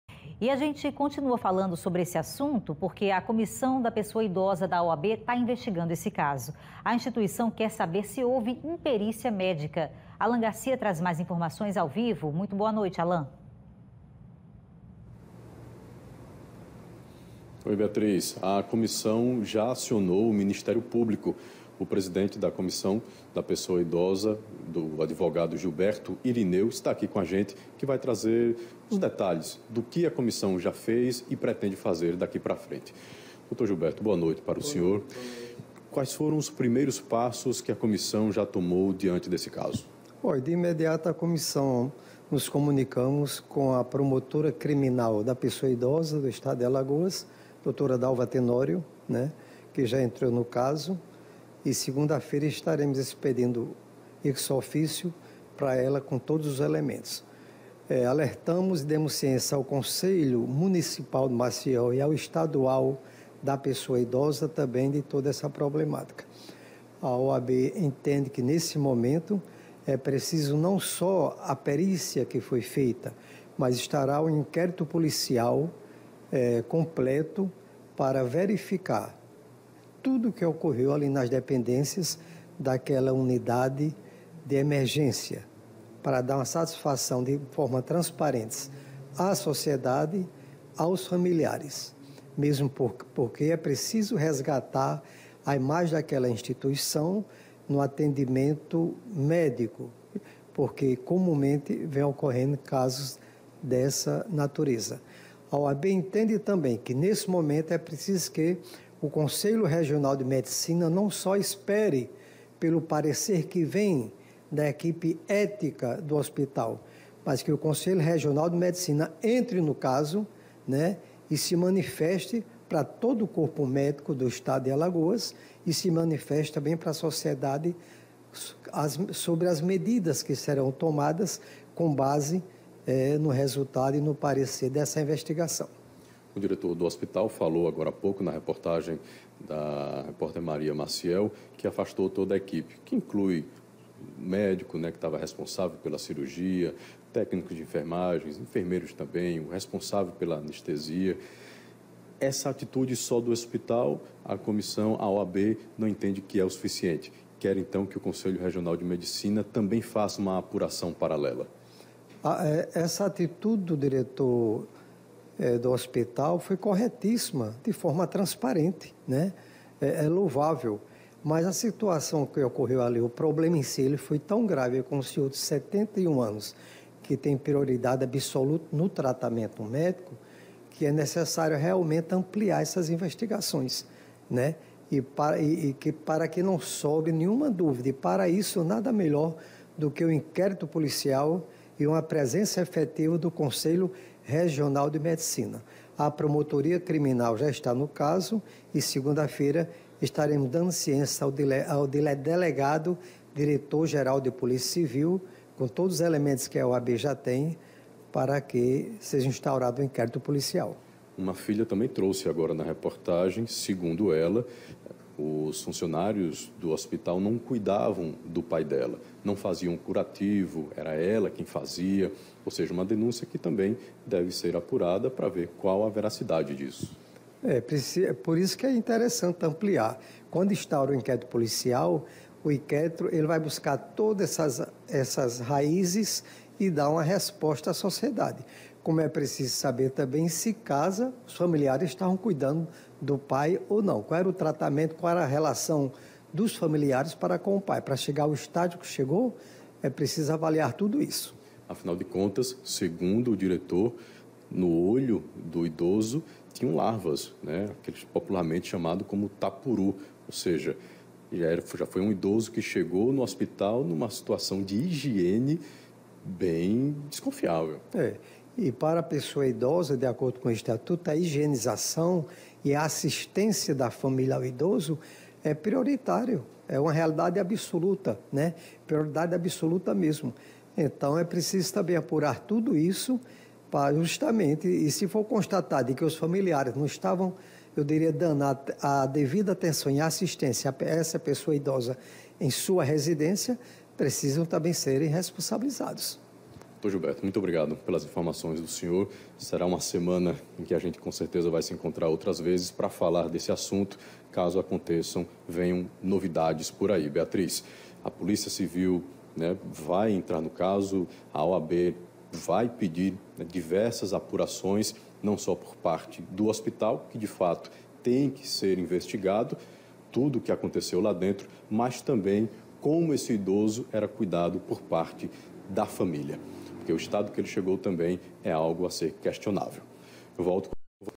OAB cobra investigação e quer saber se houve imperícia da equipe médica
Idoso queimado durante cirurgia no HGE continua em estado grave; equipe médica foi afastada